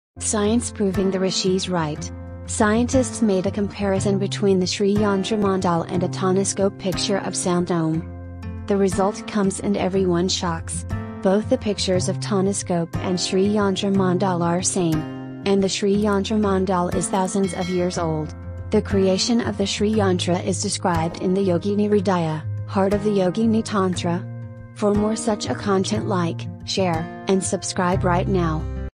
Science proving the Ancient Bharat Rishis Right | Shri Yantra shriyantra 😮
Science proving the rishis right

Scientists made a comparison between the Shri yantra mandala and a tonoscope picture of sound om.

The result comes and everyone shocks! Both the pictures of tonoscope and Shri yantra mandala are same .

And the Shri yantra mandala is thousands of years old .

The creation of the Shri Yantra is described in the Yogini Hridaya (Heart of the Yogini Tantra), which still does not exist in an English translation, as far as we are aware.

Shri yantra mandal
tonoscope images of OM
ancient Bharat rishis